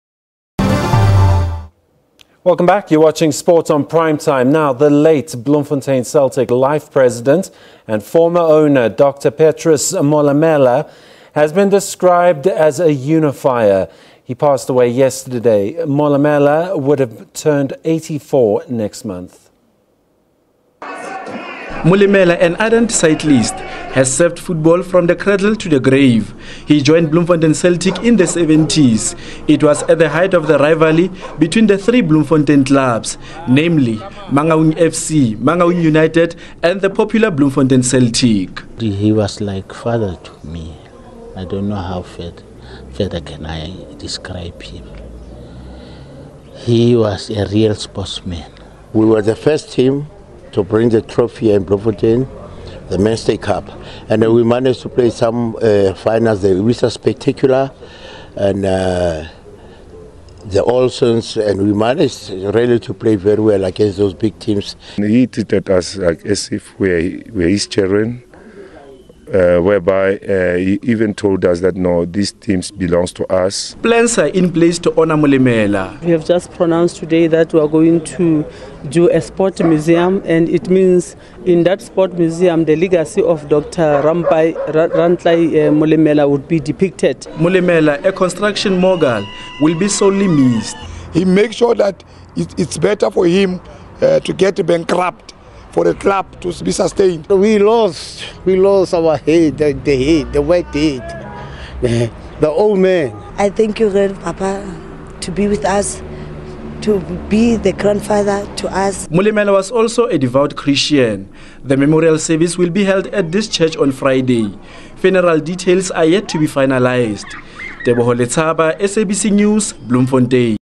Dr Petrus Molemela described as a "unifier"
The late Bloemfontein Celtic life president and former owner Dr Petrus Molemela has been described as a "unifier". He passed away yesterday. Molemela would have turned 84 next month.